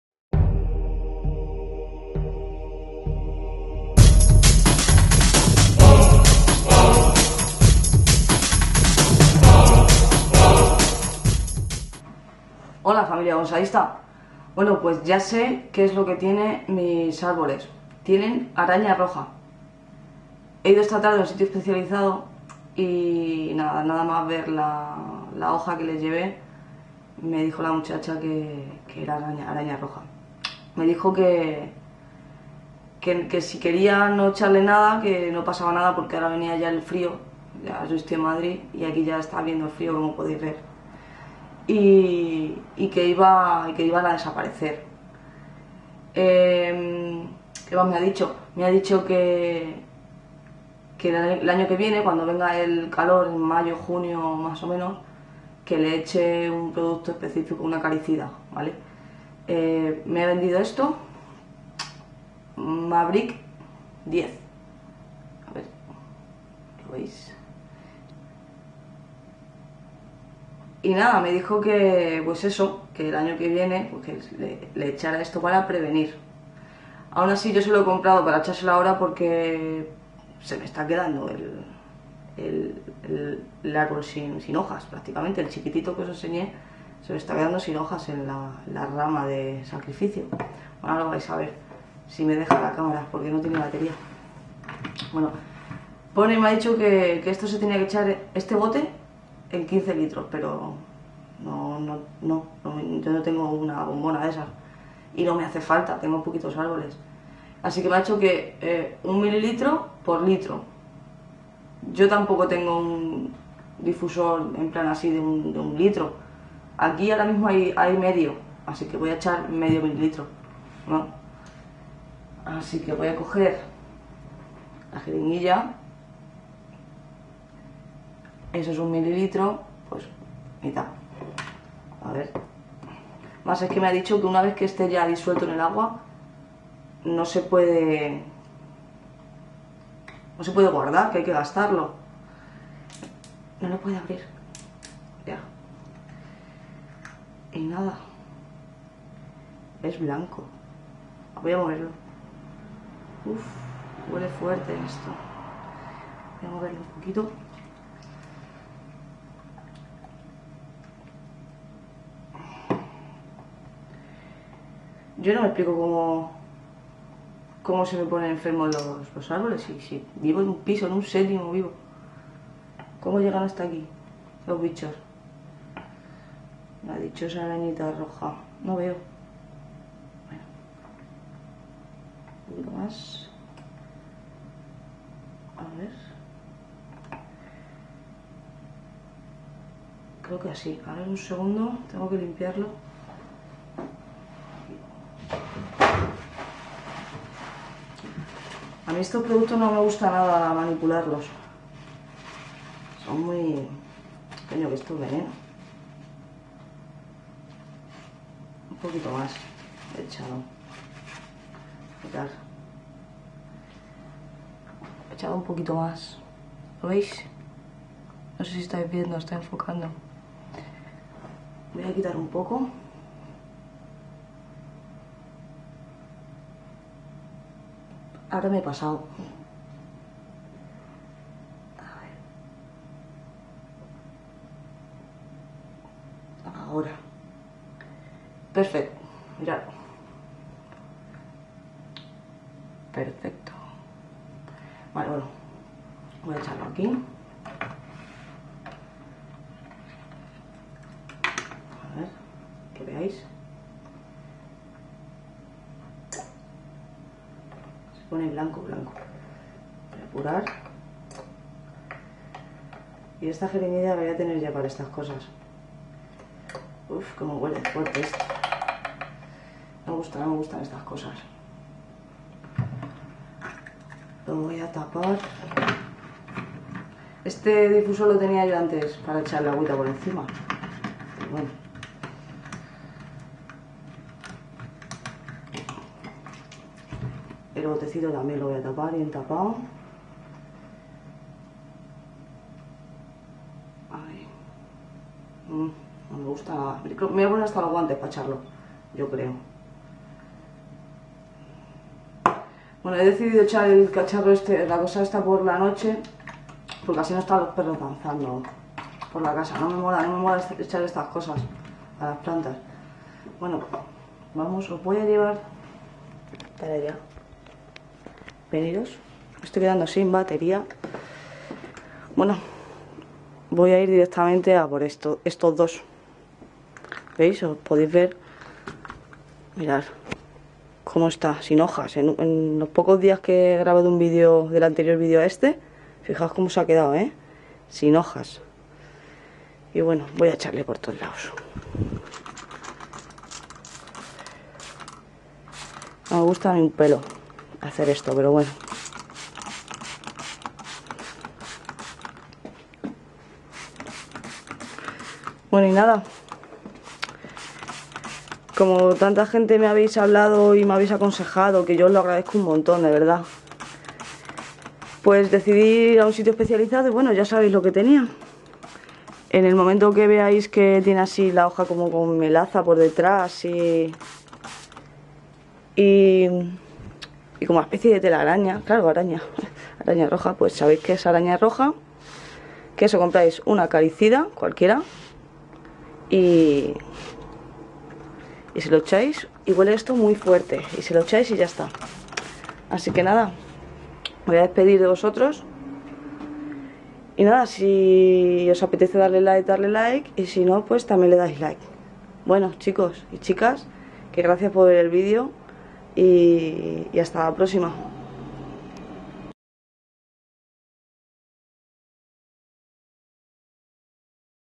que tienen mis bonsais
Hola familia!!, pues aquí os dejo un nuevo video en el que os explico lo que les pasa a mis bonsais.